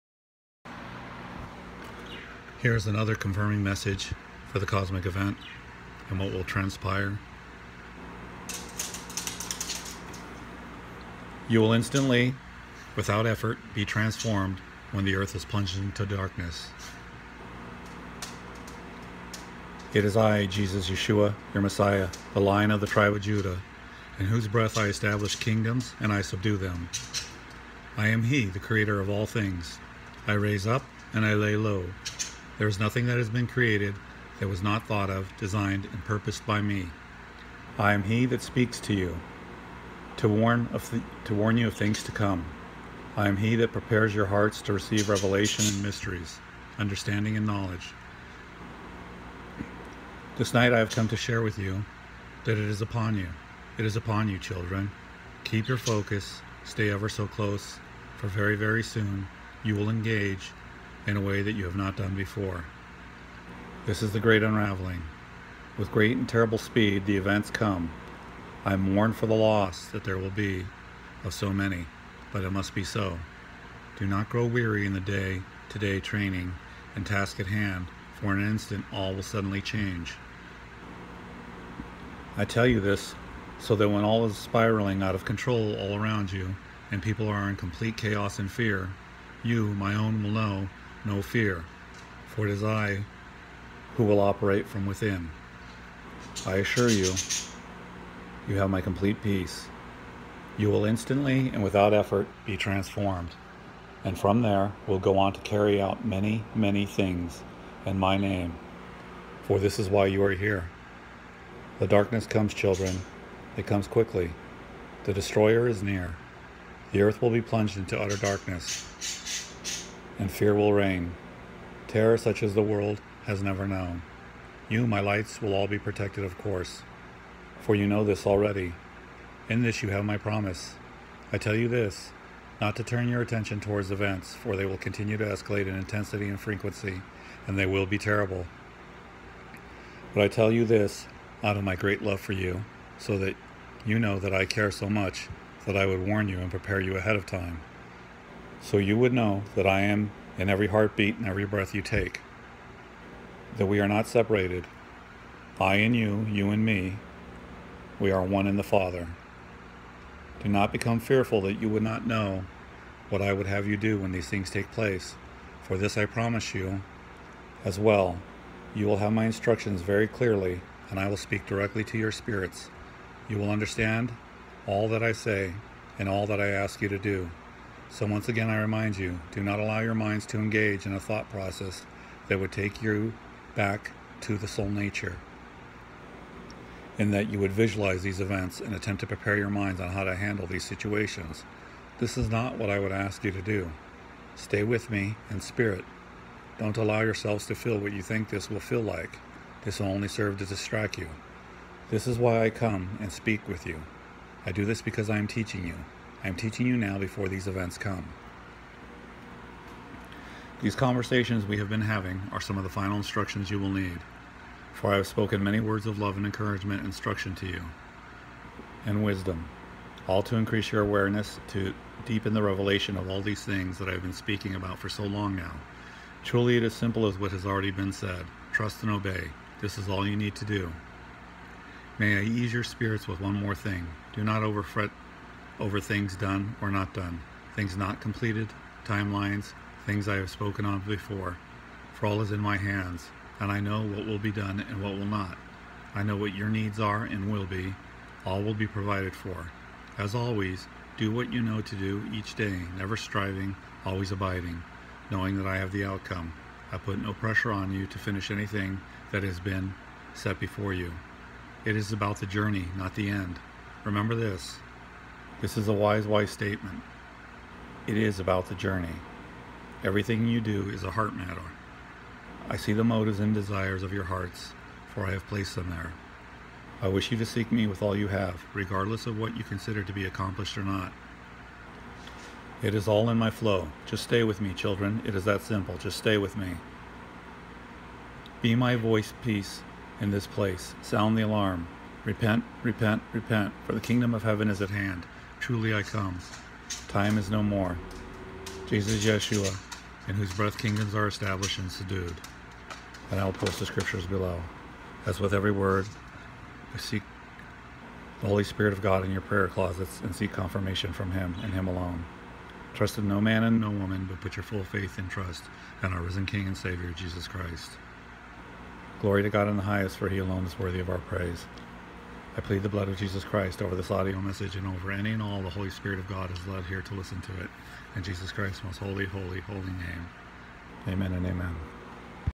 Cosmic Event Confirming Messages 6
YOU WILL INSTANTLY AND WITHOUT EFFORT BE TRANSFORMED WHEN THE EARTH IS PLUNGED INTO DARKNESS
It is I Yahushua, your Messiah, the Lion of the Tribe of Judah in whose breath I establish kingdoms and I subdue them. I am He the Creator of all things. I raise up, and I lay low. There is nothing that has been created that was not thought of, designed, and purposed by Me. I am He that speaks to warn you of things to come. I am He that prepares your hearts to receive revelation and mysteries, understanding and knowledge.

This night I have come to share with you that it is upon you. It is upon you children! Keep your focus, stay ever so close for very, very soon you will engage in a way that you have not done before. This is the great unraveling. With great and terrible speed the events come. I mourn for the loss that there will be of so many but it must be so. Do not grow weary in the day to day training and tasks at hand for in an instant all will suddenly change. I tell you this so when all is spiraling out of control all around you and people are in complete chaos and fear, you, My own, will know no fear for it is I who will operate from within. I assure you, you will have My complete peace. You will instantly and without effort be transformed, and from there will go on to carry out many, many things in My name for this is why you are here.

The darkness comes children, it comes quickly. The Destroyer is near. The earth will be plunged into utter darkness and fear will reign. Terror such as the world has never know. You My lights will all be protected, of course,  for you know this already. In this, you have My promise. I tell you this, not to turn your attention towards events for they will continue to escalate in intensity and frequency and they will be terrible. But I tell you this out of My great love for you so that you know that I care so much that I would warn you and prepare you ahead of time. So you would know that I am in every heartbeat and every breath you take, that We are not separated. I in you, you in Me, We are One in the Father.

Do not become fearful that you would not know what I would have you do when these things take place for this I promise you as well, you will have My instruction very clearly and I will speak directly to your spirits. You will understand all that I say, and all that I ask you to do. So once again,  I remind you, do not allow your minds to engage in a thought process that would take you back to the soul nature, in that you would visualize these events and attempt to prepare your minds on how to handle these situations. This is not what I would ask you to do. Stay with Me in spirit. Don't allow yourselves to feel what you think this will feel like. This will only serve to distract you. This is why I come and speak with you. I do this because I am teaching you. I am teaching you now before these events come.

These conversations We have been having are some of the final instructions you will need. For I have spoken many words of love and encouragement and instruction to you, wisdom; all to increase your awareness, to deepen the revelation of all these things that I have been speaking about for so long now. Truly, it is as simple as what has already been said, trust and obey. This is all you need do.

May I ease your spirits with one more thing, do not fret over things done or not done, things not completed, timelines, things I have spoken of before. For all is in My hands and I know what will be done and what will not, I know what your needs are and will be. All will be provided for. As always do what you know to do each day, never striving, always abiding, knowing that I have the outcome. I put no pressure on you to finish anything that has been set before you, it is about the journey, not the end. Remember this, this is a wise, wise statement. It is about the journey.

Everything you do is a heart matter. I see the motives and desires of your hearts for I have placed them there. I wish to see you seek Me with all you have, regardless of what you consider to be accomplished or not. It is all in My flow. Just stay with Me children, it is that simple, just stay with Me.

Be My voice-piece in this place, sound the alarm!

REPENT! REPENT! REPENT!

For the Kingdom of Heaven is at hand! Truly I come!!

*TIME IS NO MORE*

YAHUSHUA --IN WHOSE BREATH KINGDOMS ARE ESTABLISHED AND SUBDUED